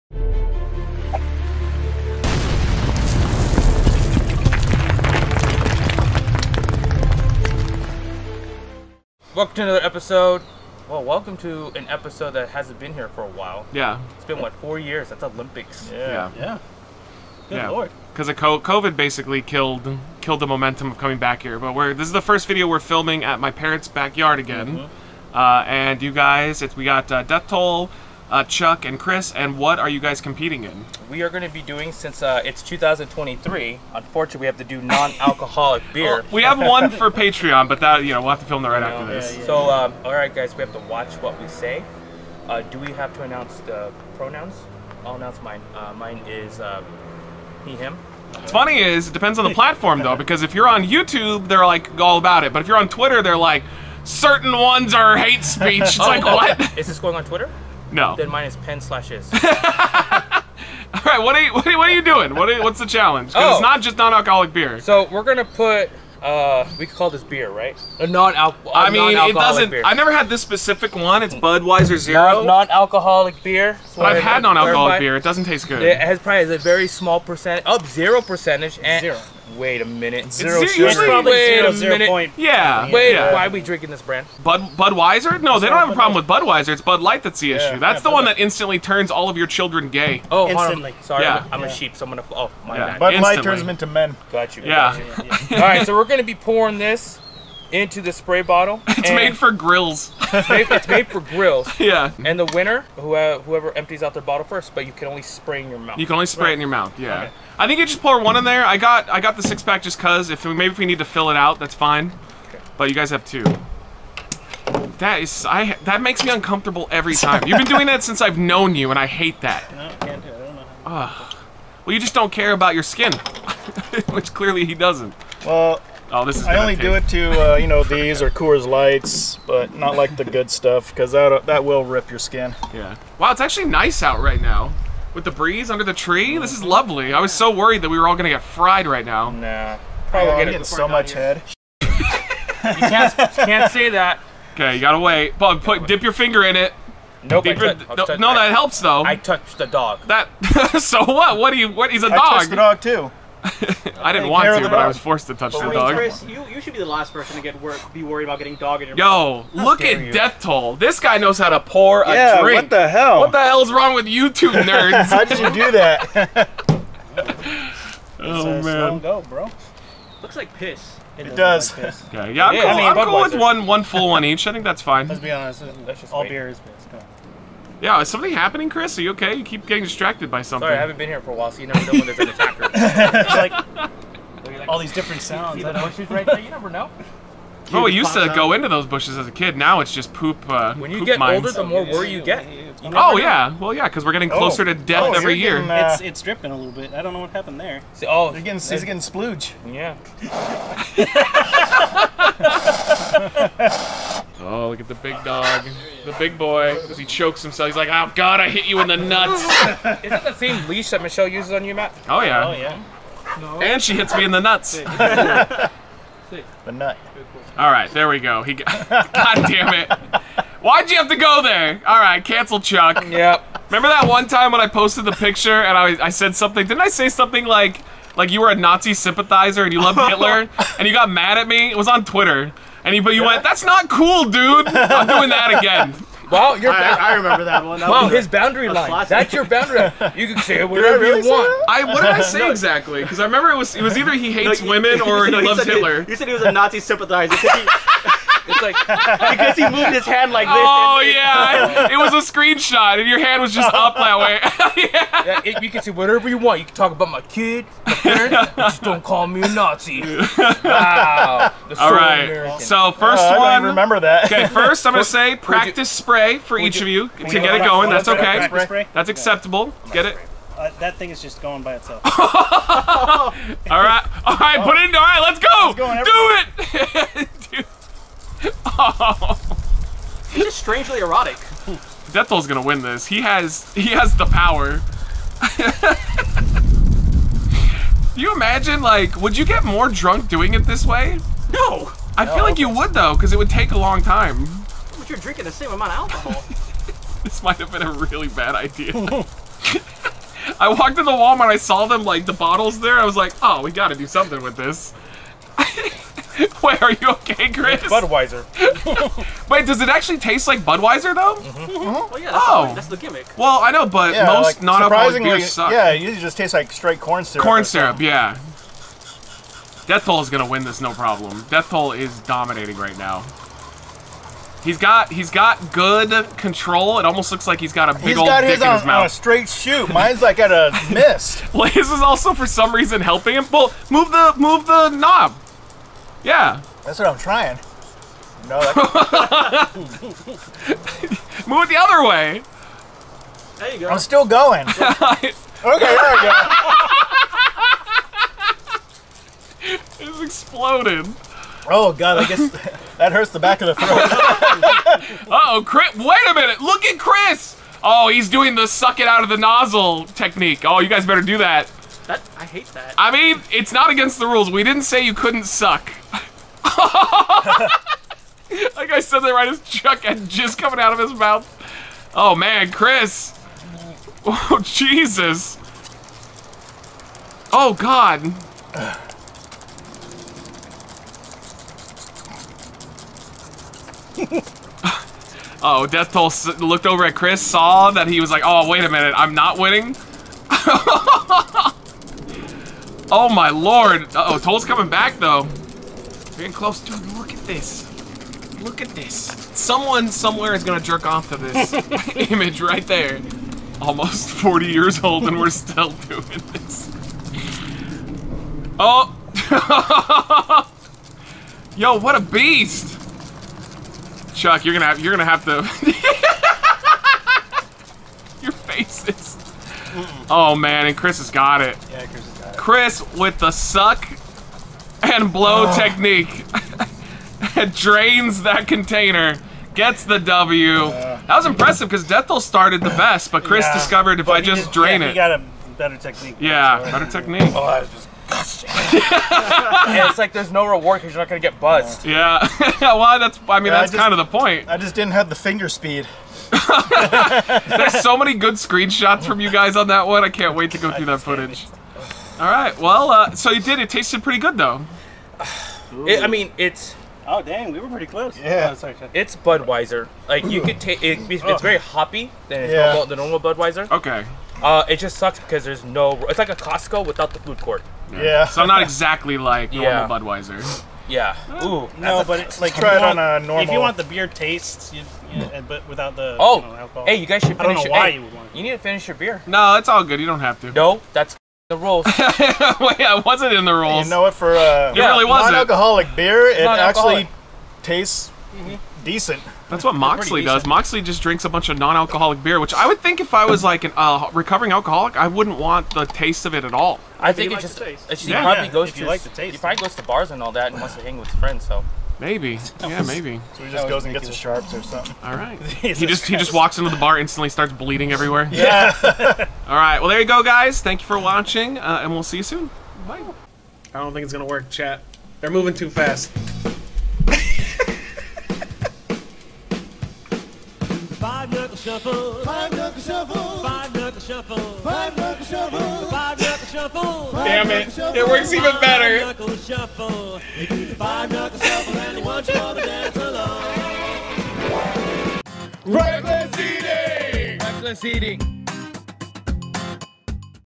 Budweiser Zero Spray Bottle Challenge
On today's Wreckless Eating episode the crew will be taking on the Budweiser Zero Spray Bottle Challenge!